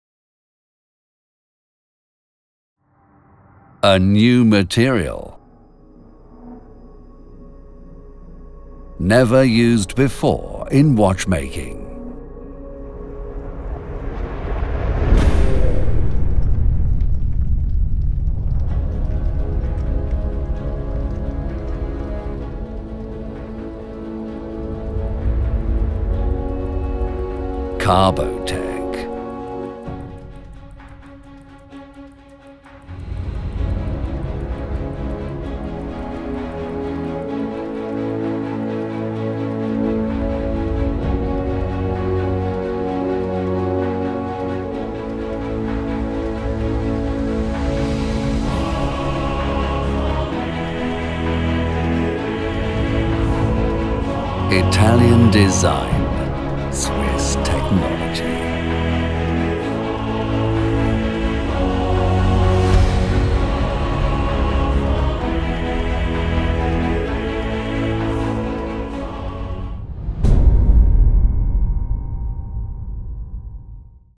Panerai Carbotech
The Luminor Submersible 1950 Carbotech™ 3 Days Automatic (PAM00616) presents a remarkable new technical innovation: the case is made of carbotech, a composite material based on carbon fibre, never before used in the world of watchmaking.